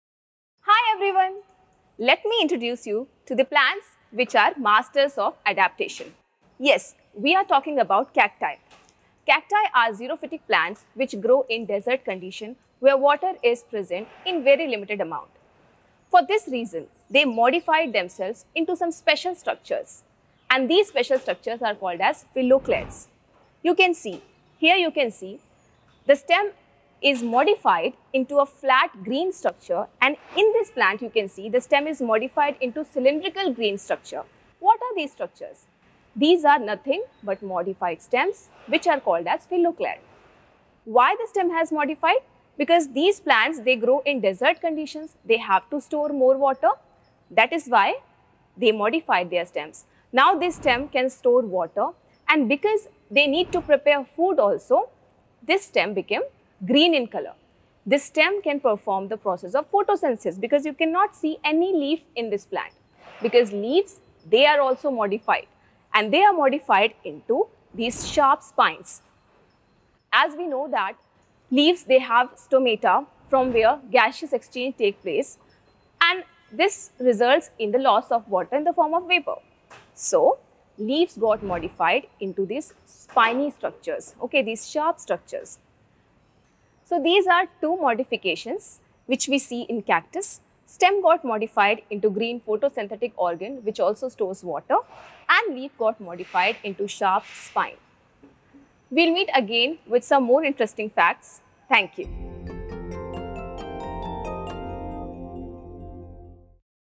Phylloclade & Spine | Modification of Stem | Biology | CBSE Online
NEET Expert and Super Teacher at Homeskul - Khushboo Bhardwaj Ma'am, handling a short portion on Modification.

The phylloclade is a special modified photosynthetic stem present mostly in xerophytes. It is a green, flattened, or cylindrical structure that has distinct nodes and internodes. Xerophytes show many adaptations to check the rate of transpiration. Reduction of leaf size, early leaf fall, formation of scale leaves, spines, thorns, thick cuticle, presence of fewer stomata is some of the xerophytic characters. In such cases, the stems become flattened to carry out photosynthesis. These modified stems are called phylloclades. Usually, the phylloclades retain water in the form of mucilage. e.g., Opuntia.

In Opuntia, the leaves are modified into spines and the stems become photosynthetic, fleshy, and leaf-like which is called phylloclade.